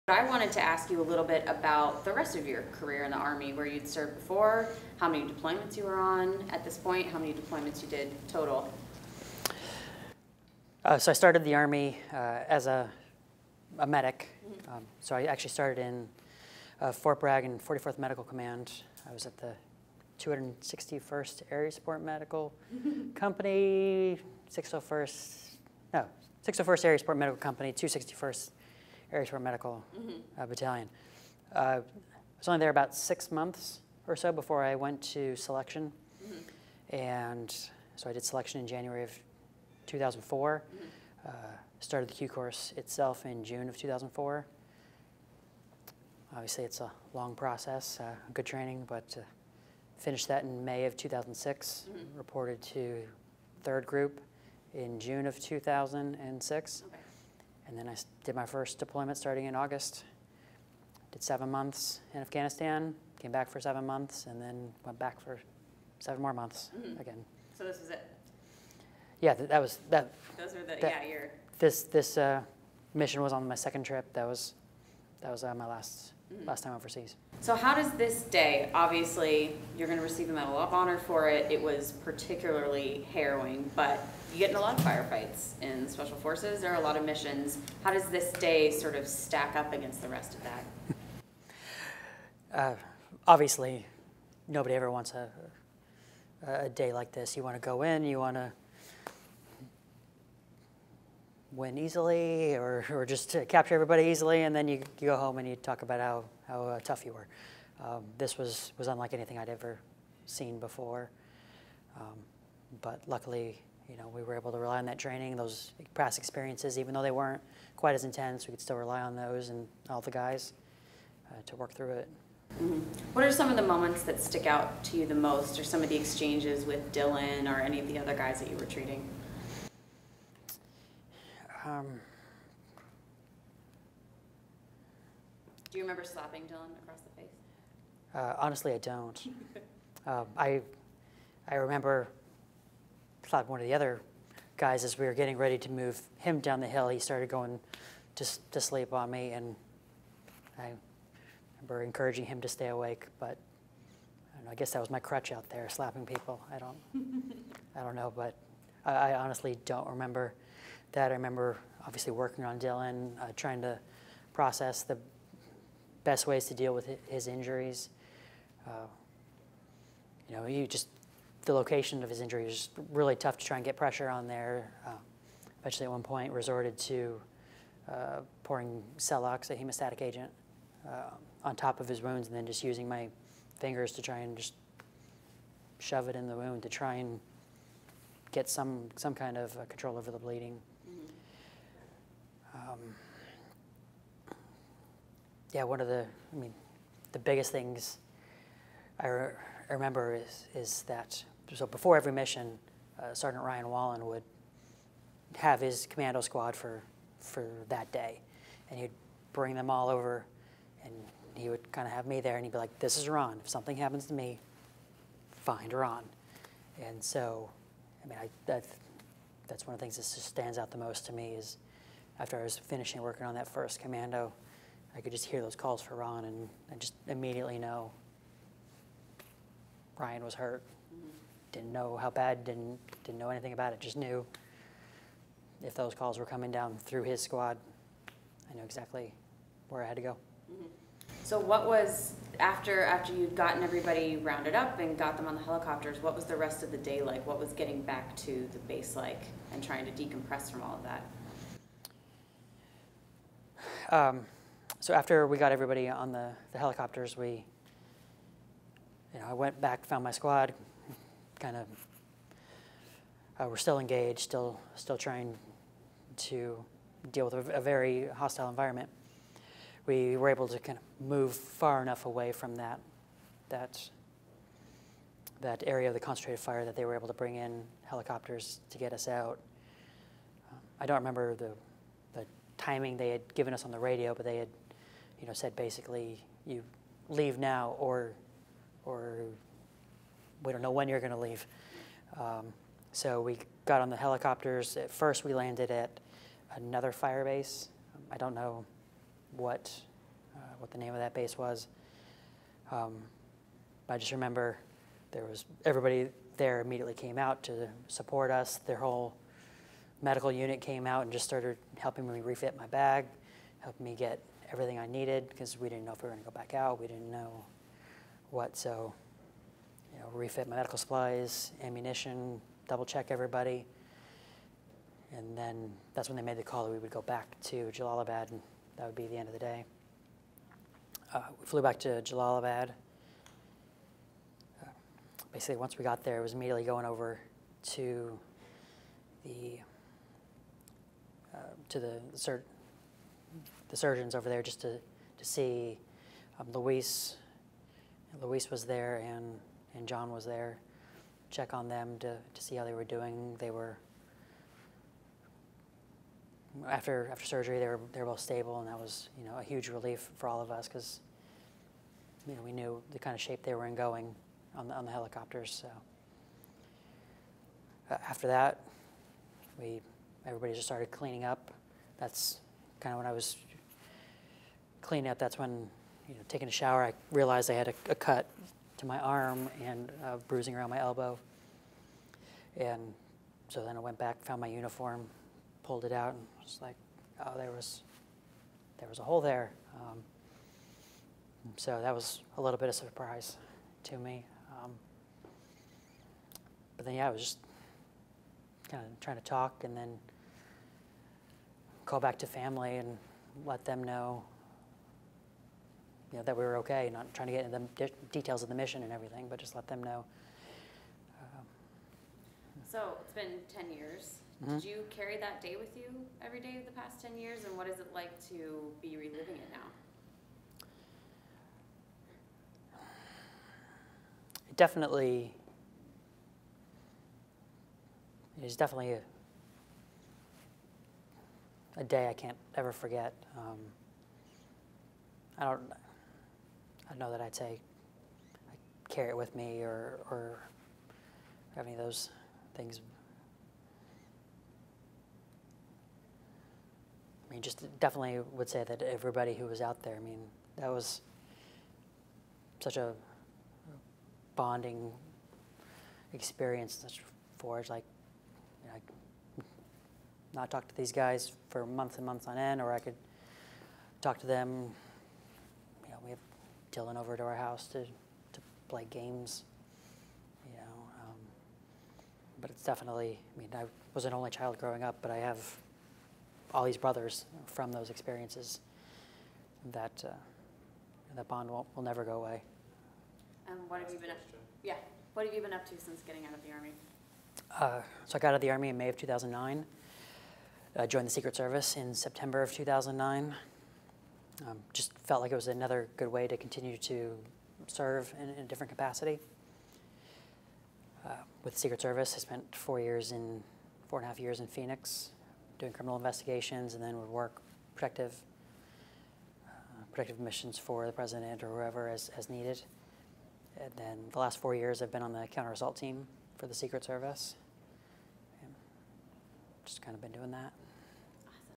Interview with Staff Sgt. Ron Shurer II, Medal of Honor recipient (2018)
Army Times' Meghann Myers interviews Medal of Honor recipient former Army Staff Sgt. Ron Shurer II. (2018)